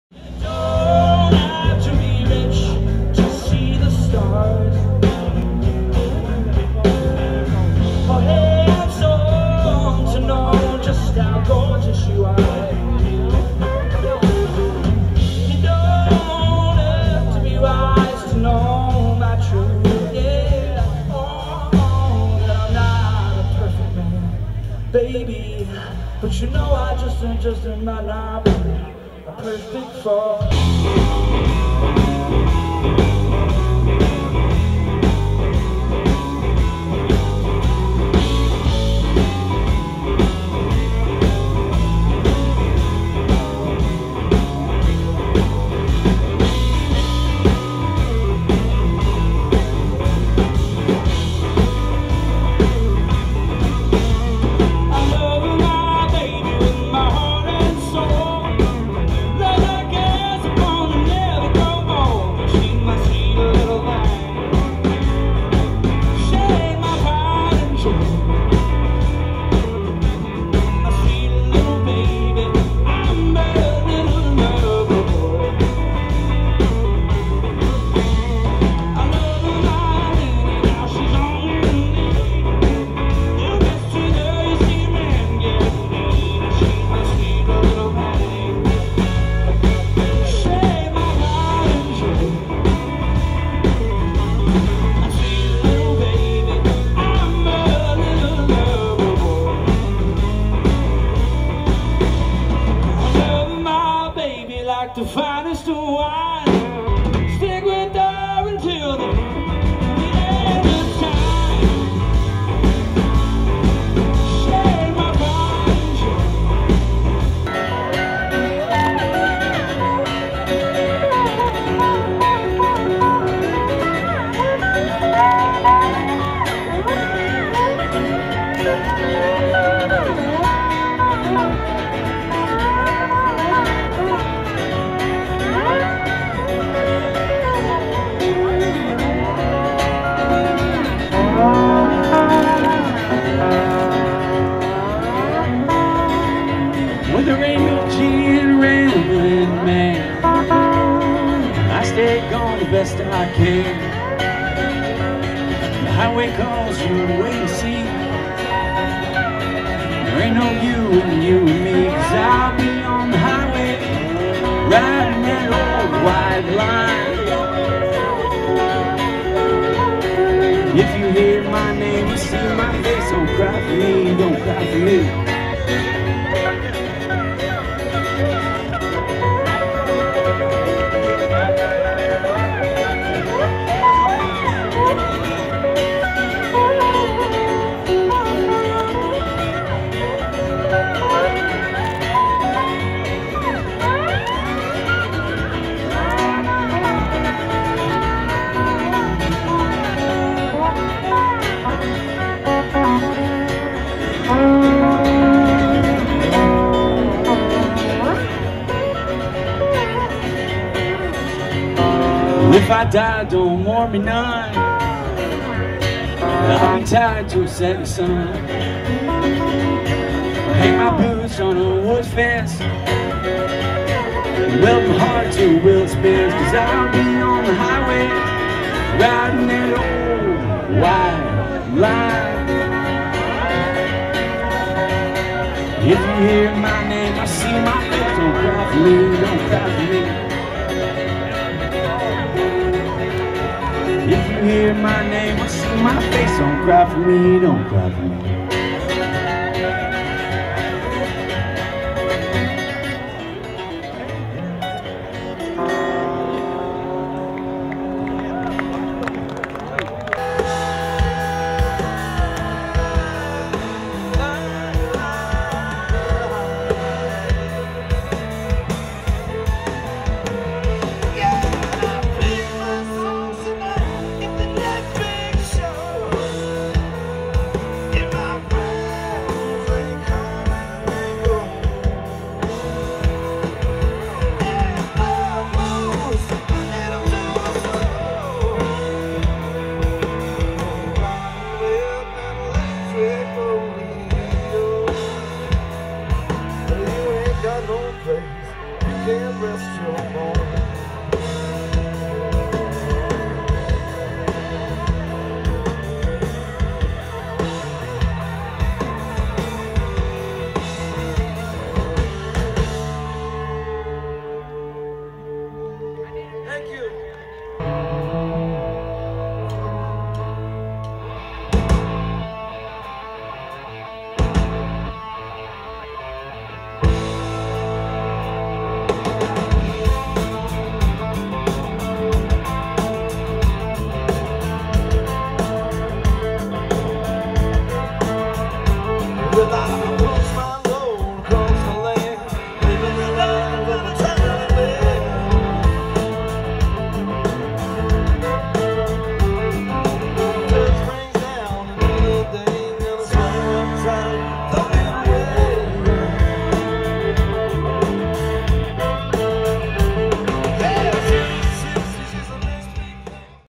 Great Am Music Fest
Some video I shot at the Great Americana Music Festival in Beaumont Texas June 10th 2017